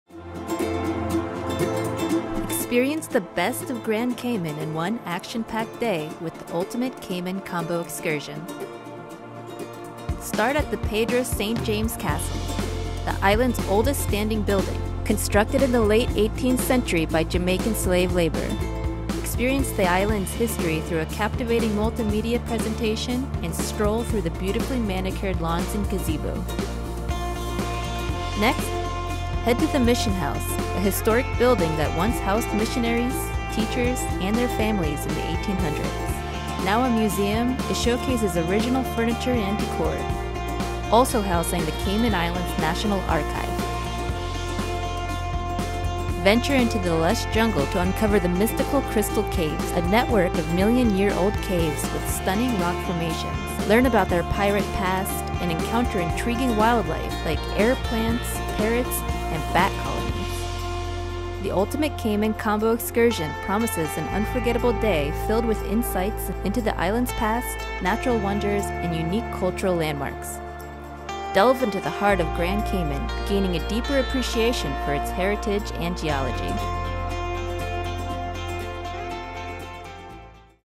Ultimate Cayman Combo Excursion - Caves, Castles and Mission
Timestamps
0:00 Experience an action packed day with the ultimate cayman tour
0:10 Start at Pedro St James
0:19 Learn about the island's history
0:29 Next, head to the historic Mission House
0:45 Explore the Crystal Caves

Wanting the best of Grand Cayman in one day? You've found it. The Ultimate Cayman Combo Excursion will take you to the Pedro St. James Castle, the Mission House and Cayman Island's Crystal Caves. You'll finish this tour with a deep appreciation for the history, geology and unique buildings of the island.

The Pedro St. James Castle is the oldest standing building on the island. Built in 1801 with slave labor from Jamaica, this peculiar three-story building has been transformed into a museum, where you can learn about the history of Grand Cayman via an incredible multimedia presentation before touring around the place, where you'll be able to admire the impeccably manicured lawns and a beautiful gazebo.

Your next stop will be the Mission House, a historic building that rose to prominence in the 1800s when it was used as a house for missionaries, teachers, and their families. The Mission House is now a beautiful museum that showcases original furniture and decor, as well as home to the Cayman Island's National Archive.

Your third, and last stop will take you deep into Cayman's jungle, where you'll trek through the lush tropical forest on your way to the Crystal Caves, million-year-old caves filled with incredible rock formations, which pirates used as hideouts back in the day. As you walk from cave to cave, you'll be able to see strangling balsam trees, all kinds of air plants, colorful parrots, and even large groups of bats that make the caves their homes.